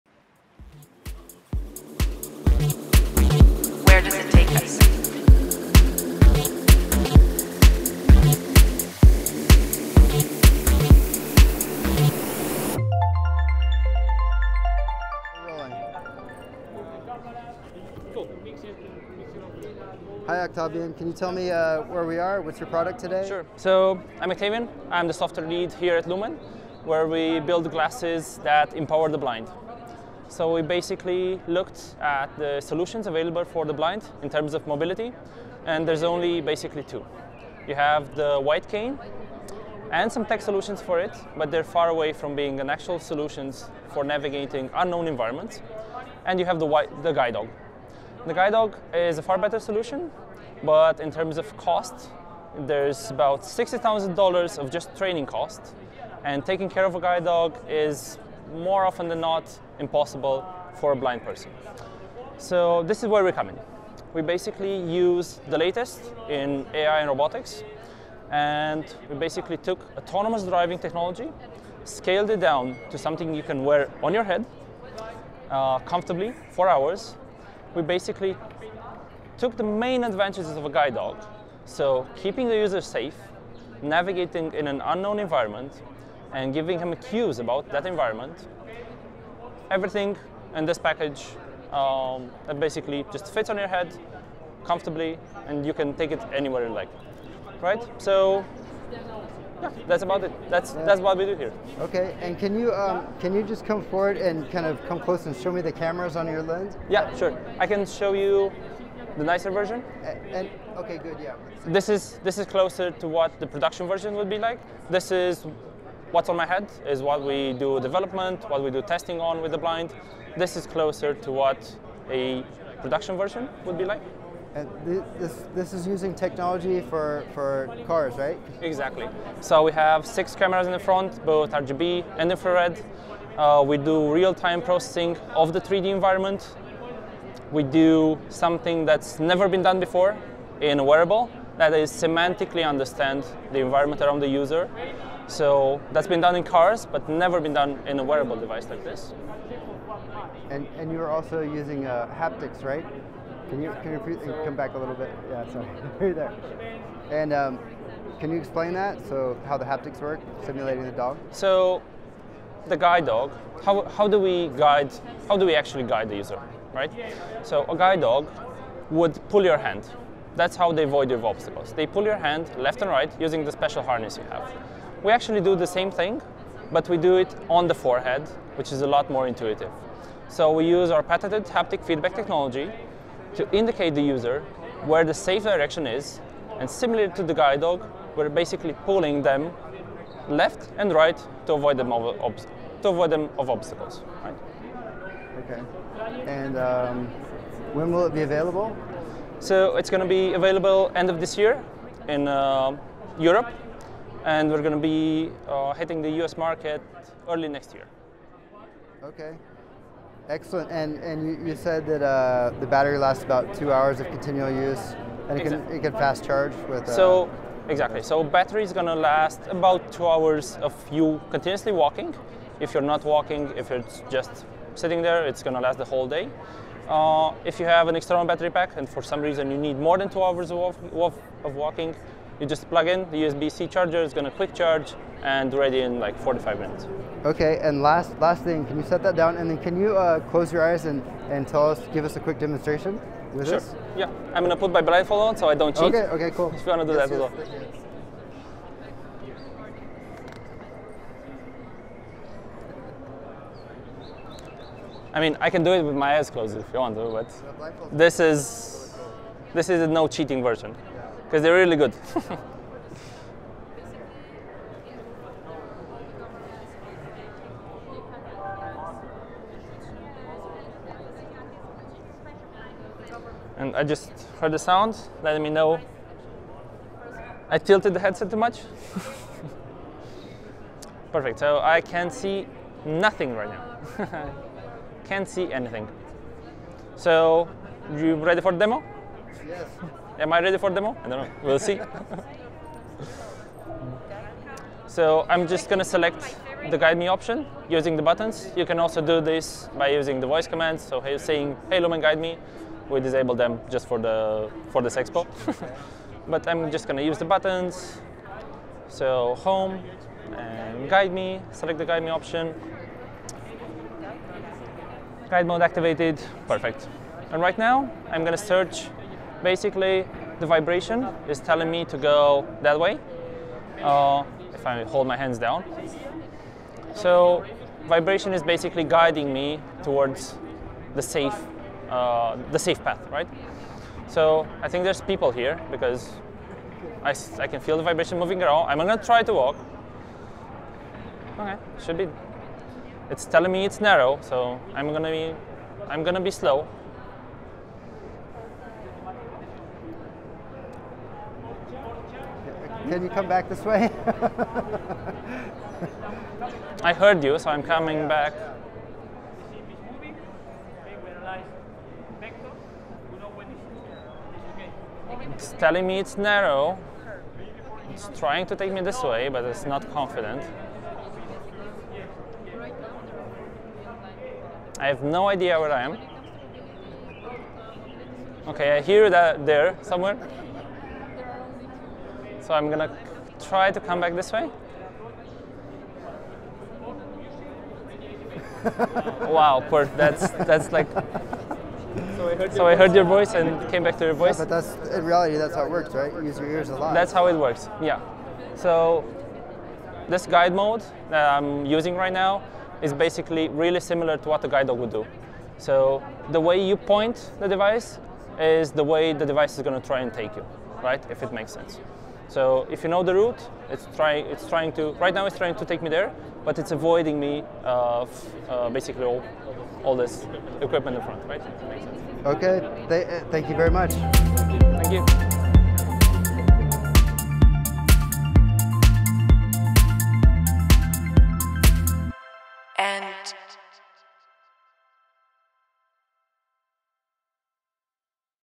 CES 2024 - Glasses for the Blind by dot Lumen - VR 180 3D Teche
CES 2024 interview with Octavian from dot Lumen who make glasses to help the blind navigate using haptics and camera sensors similar to the way self driving cars function.  Watch this video in 3D with a VR headset for the best viewing experience.